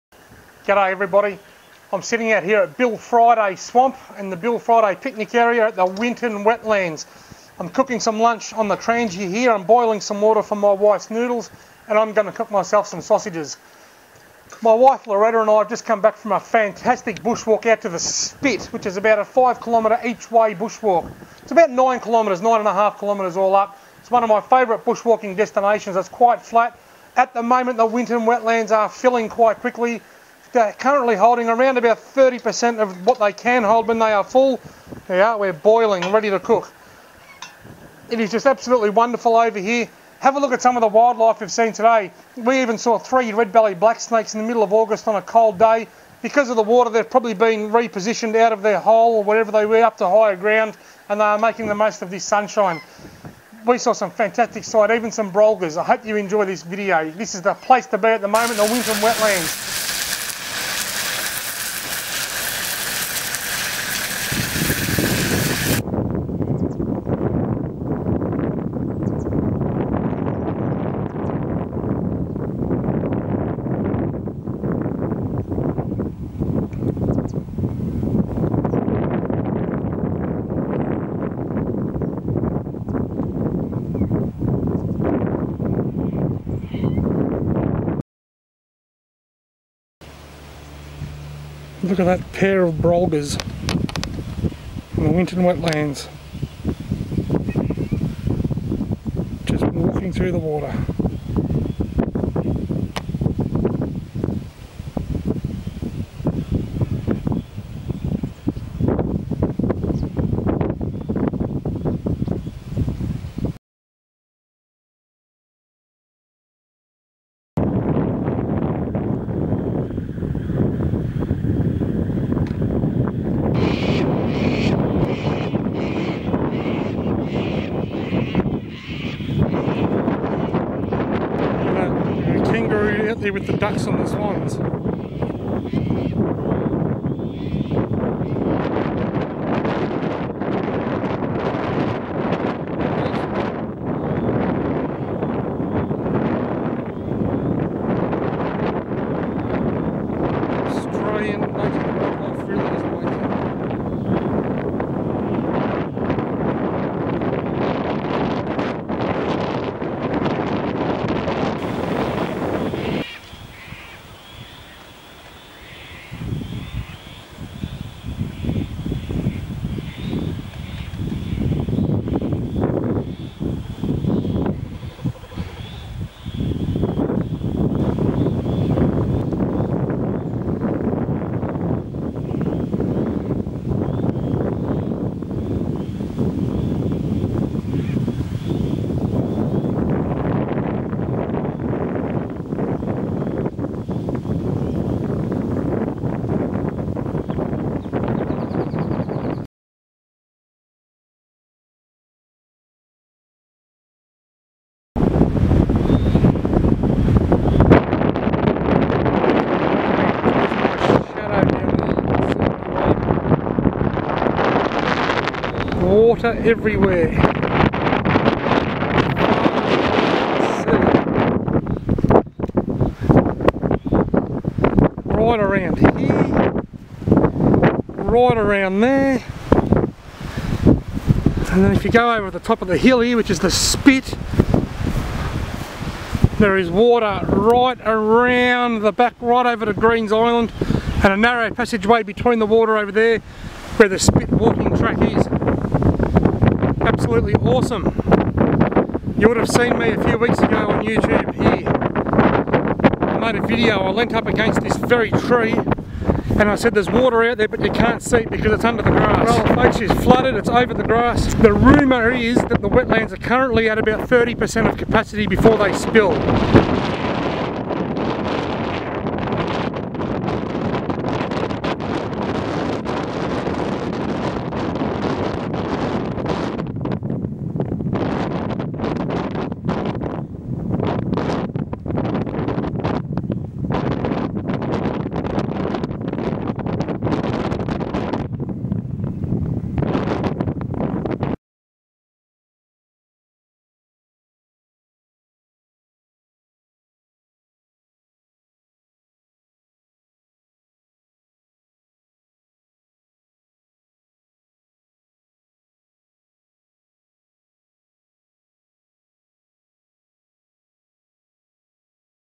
Australian wildlife in the 'now filling' Winton Wetlands
Australia has some of the most amazing wildlife in the whole world and the Winton Wetlands is one of the best places to go and observe native Australian animals in their natural environment.
 At the moment the Winton Wetlands are filling very quickly. My wife and I went hiking out there this morning and saw some spectacular wildlife.
 Here is some video footage and photos from the Winton Wetlands this morning. I apologise about the wind, but I was only carrying my basic filming equipment on the 10km hike.